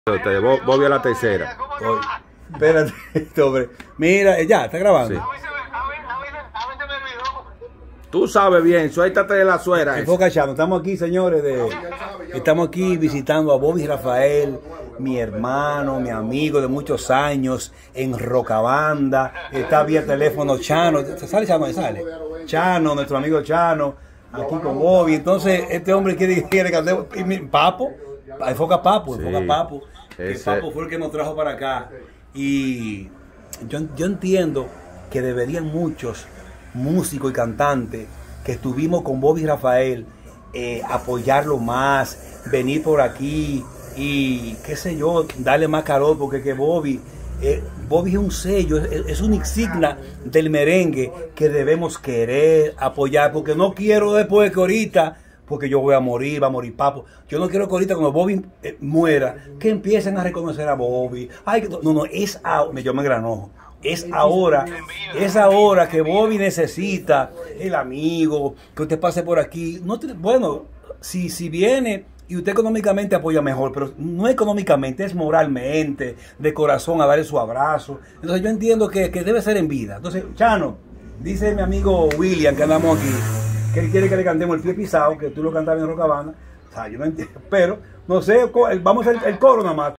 Raffy Diaz visita a Bobby Rafael (La Bobina) Ex Rokabanda para conocer su condición de salud.
Video de Raffy Diaz